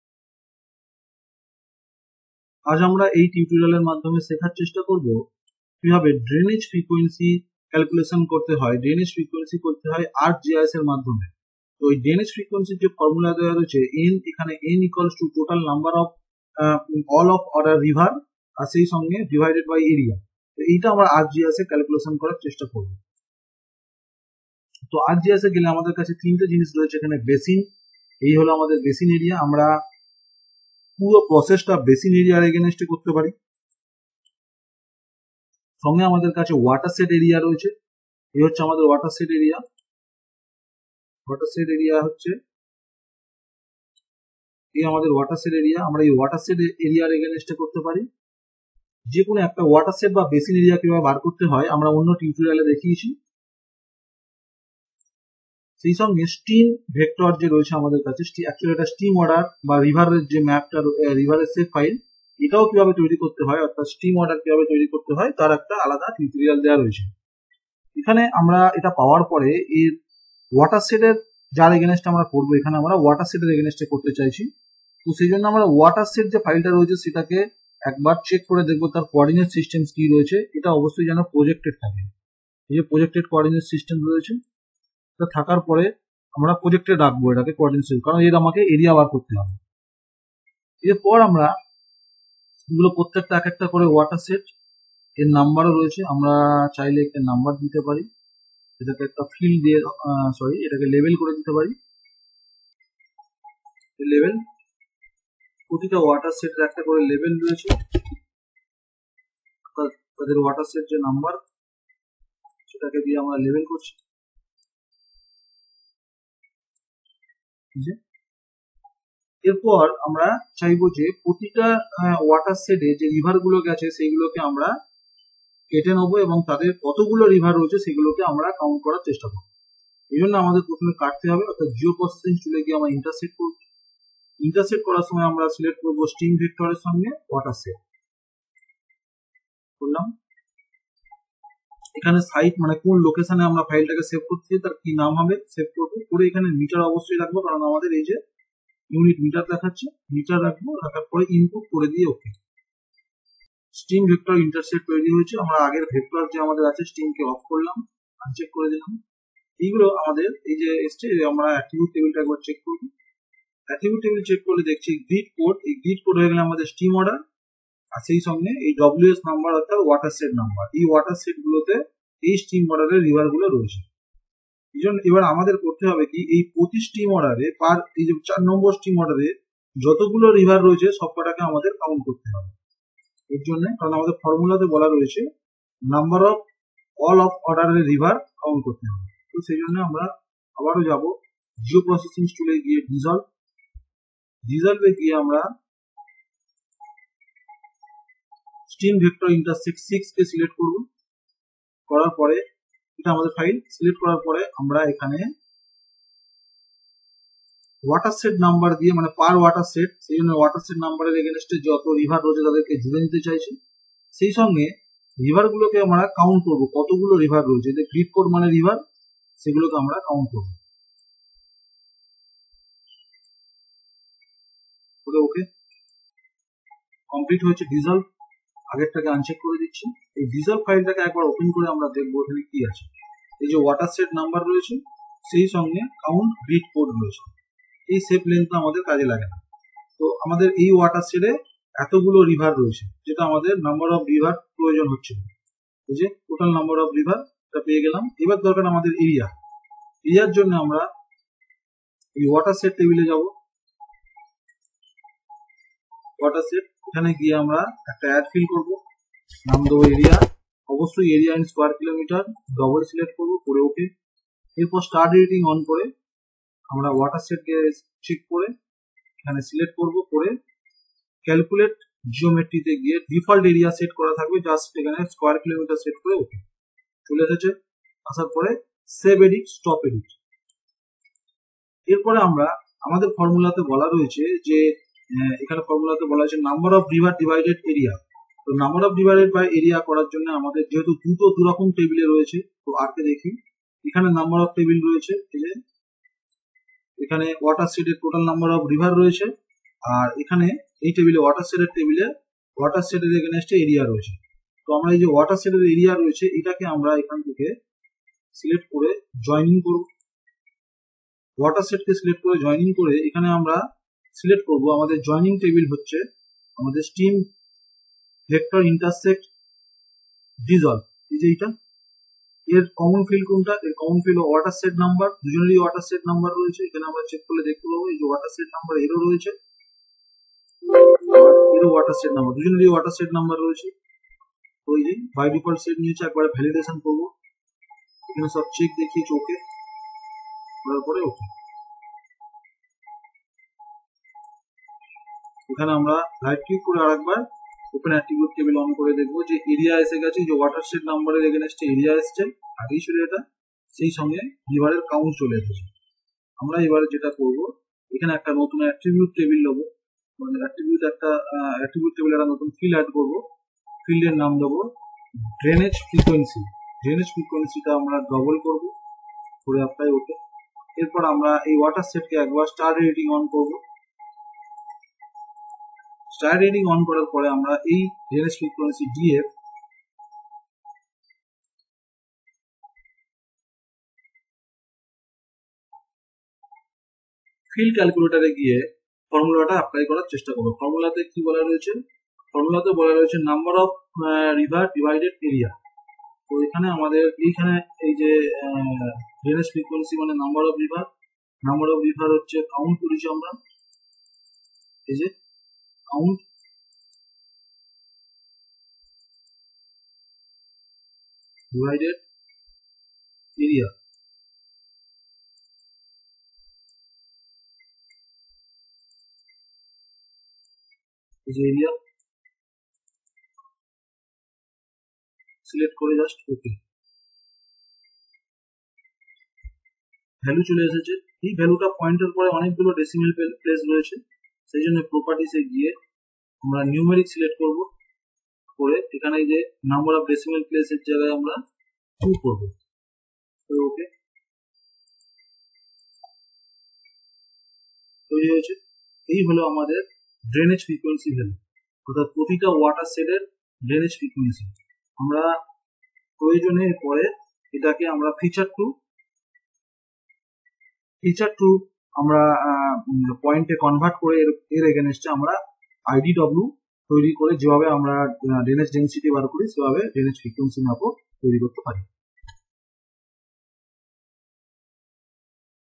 Stream Frequency Mapping in ArcGIS || Drainage Frequency || GIS & RS Oplossing in Bengali
This video is about the stream frequency mapping. And use of line density tools for the calculation of stream frequency. In previous lecture we discuss about the stream order map by arcmap. In this lecture i show you that how to find the stream frequency of the drainage basin follow me step by step and do comment on lectures.